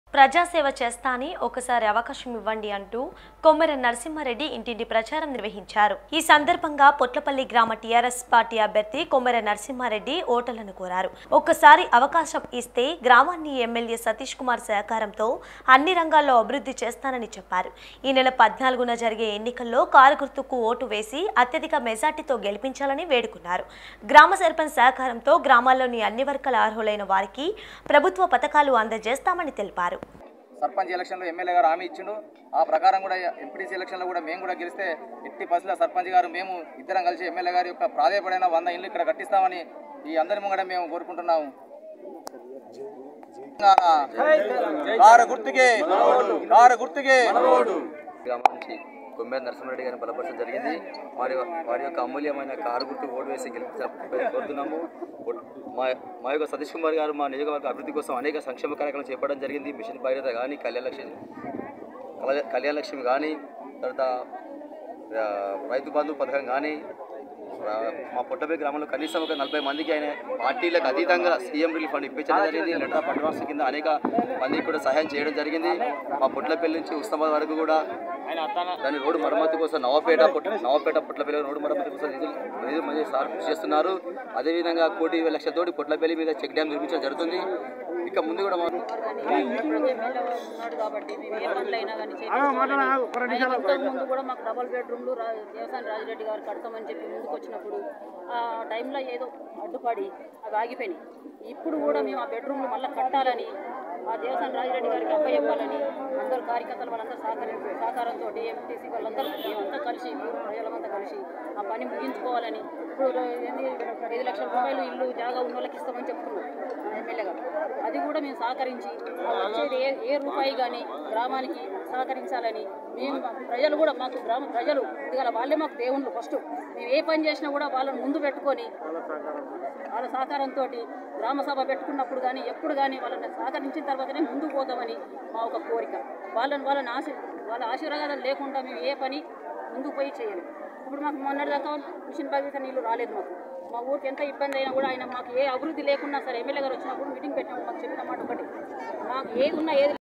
TS24 News ప్రజాసేవ చేస్తా ఒక్కసారి అవకాశం ఇవ్వండి - కొమ్మేర నర్సింహారెడ్డి
ప్రజాసేవ చేస్తా ఒక్కసారి అవకాశం ఇవ్వండి - కొమ్మేర నర్సింహారెడ్డి